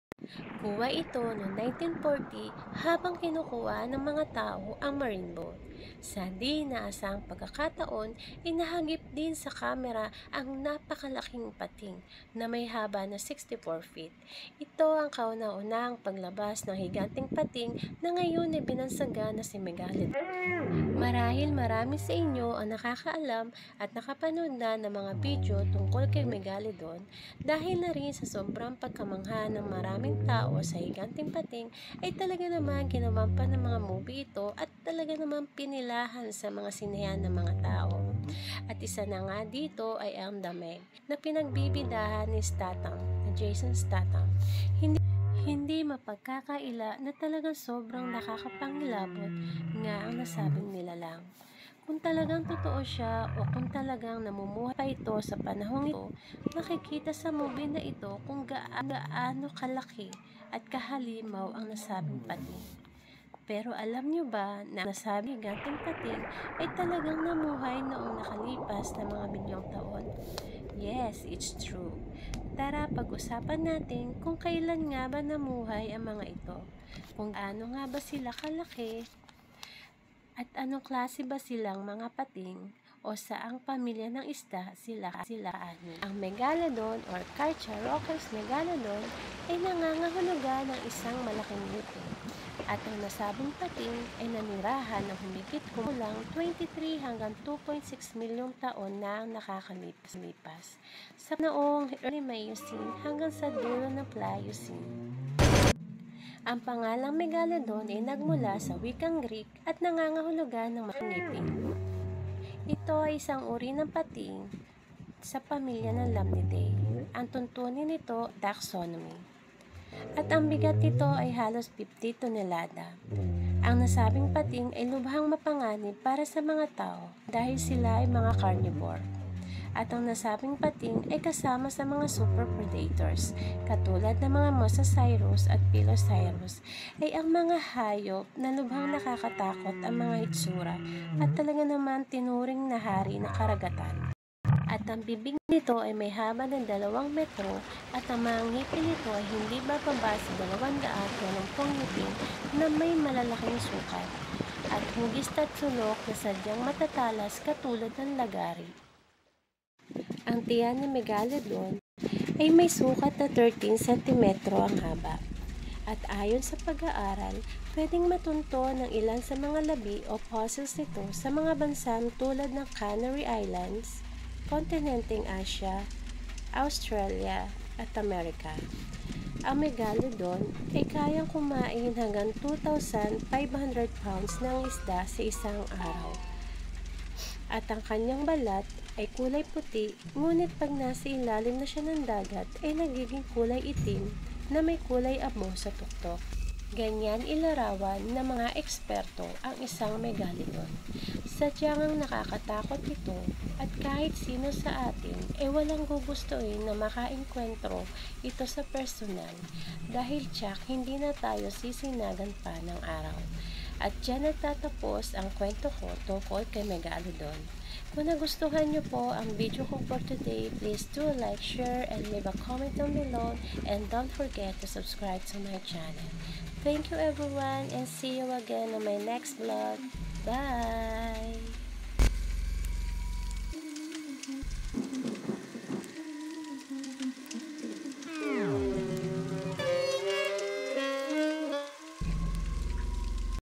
MEGALODON THE PRE-HISTORIC GIANT SHARKS I THE LIFE OF MEGALODON I THE LARGEST SHARK IN HISTORY
megalodon

Today I'm going to tell the story about the largest shark in history. Megalodon is a very large sharks that no one can't even imagine, And if you want to know about megalodon just watch my video and listen to my story.